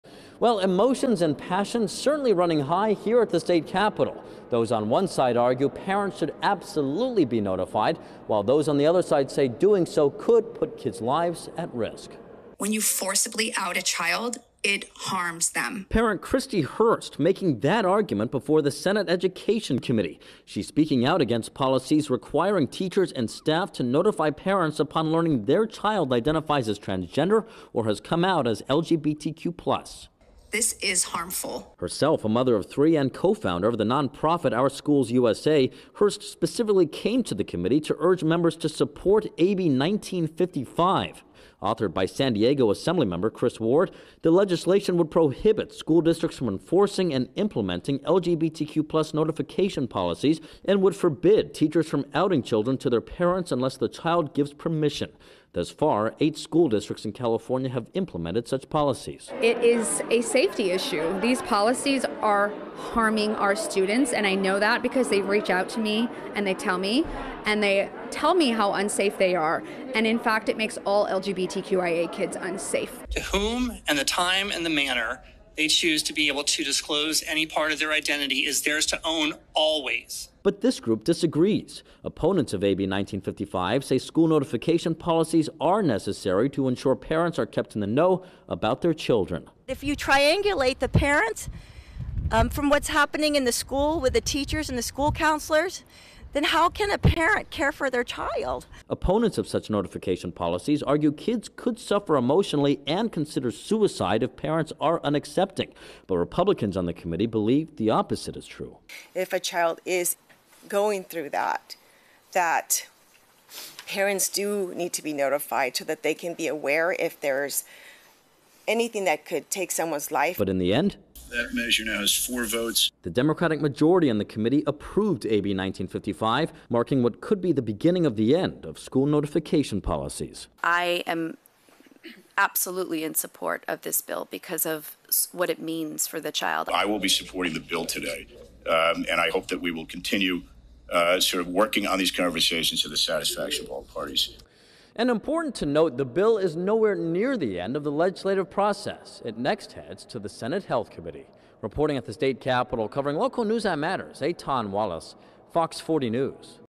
Proposed bill would forbid school districts from creating, enforcing LGBTQ+ notification policies
It was a contentious day at the State Capitol where lawmakers took up a bill that would prohibit school districts from notifying parents if their children identify as transgender or come out as LGBTQ+.